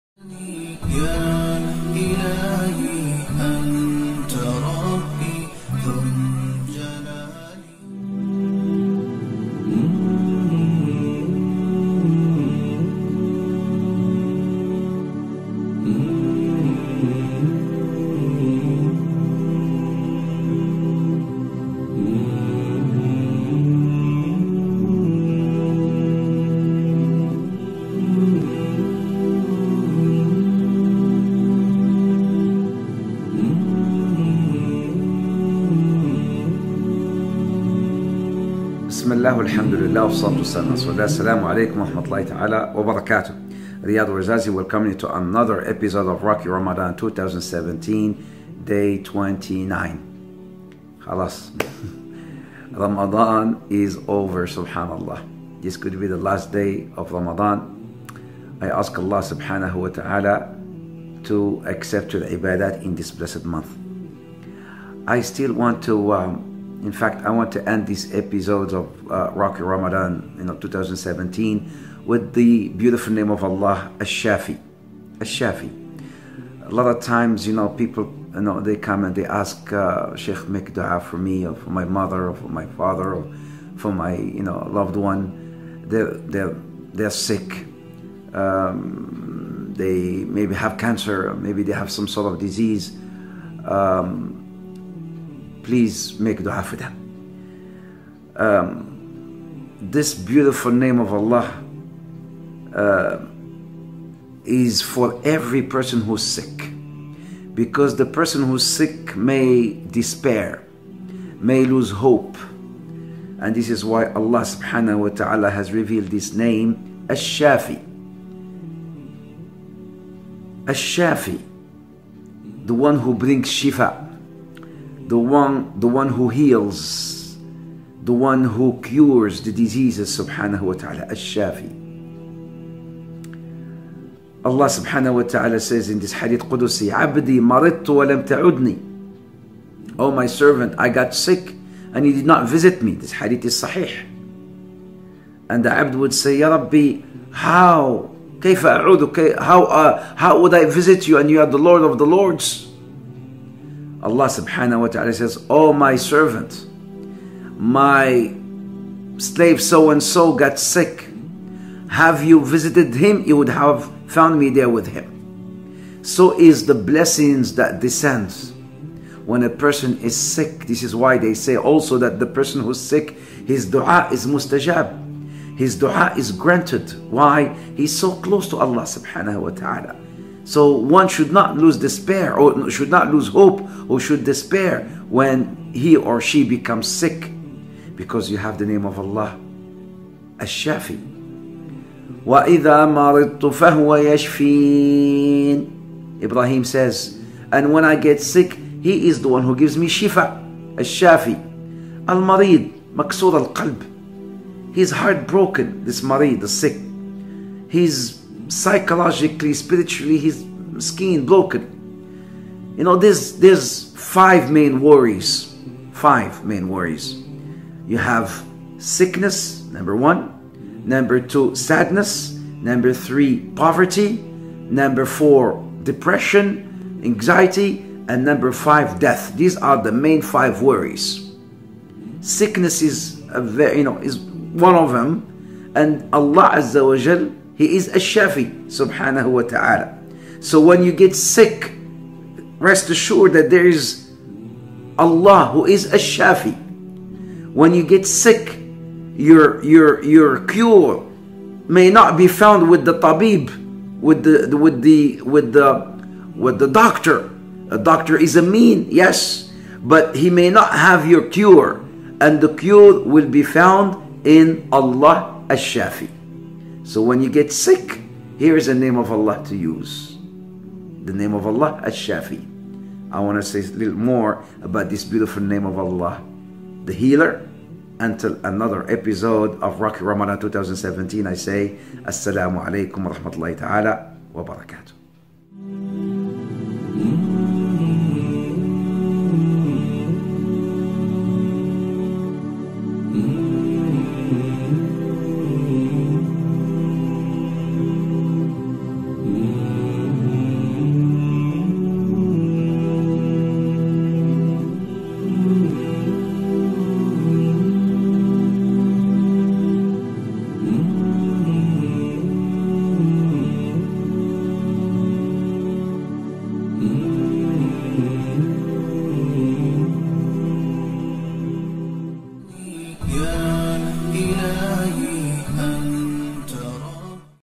ROC YOUR RAMADAN 2017 | Day 29 | Daily Reminders |Ash-Shaafee— The One who cures, the Healer
Daily Reminder Day 29 - Ash-Shaafee— The One who cures, the Healer. Believe Allah swt is the only Healer and that Allah swt is the One who heals whomsoever He wills and He decrees sickness and death for whomsoever He wills. So trust in Him first and foremost,  and then make use of the lawful treatments He made available for you through medical and other sciences. However, never attribute healing to medicines or other means and always to Ash-Shaafee only. Don’t forget to say say bismillah when you take any medicine.